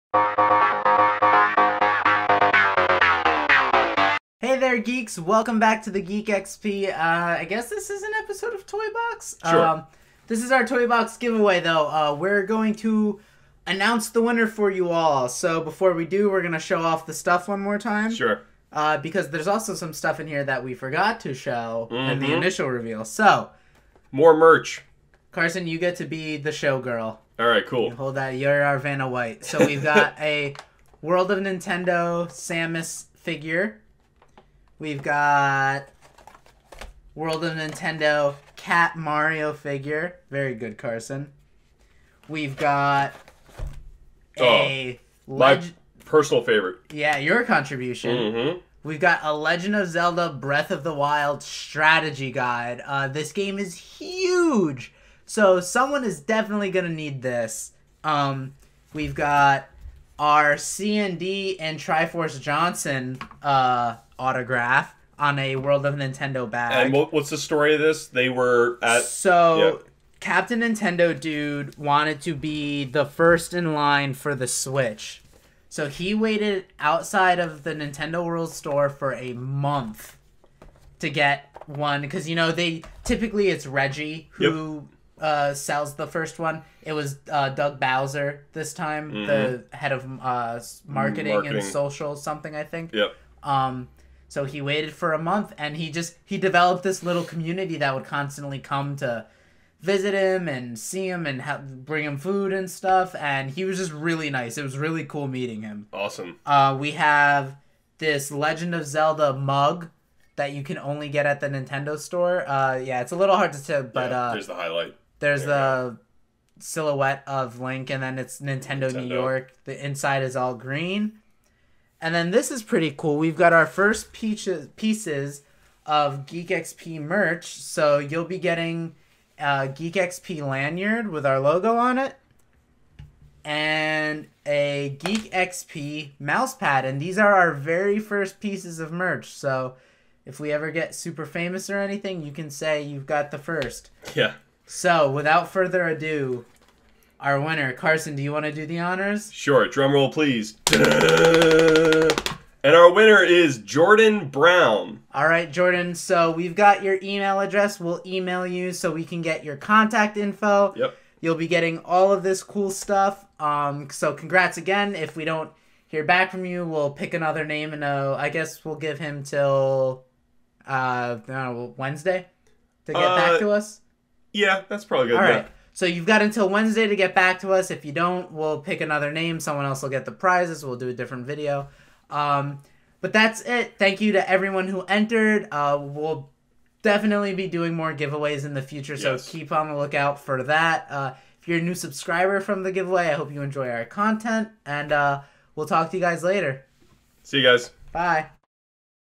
Nintendo Giveaway Winner Announced!!
Hey Geeks! The winner has been selected and we will be sending out a email to get mailing info. Thanks you to everyone that joined us for this fun giveaway. We really appreciate people voting and following us on various social media.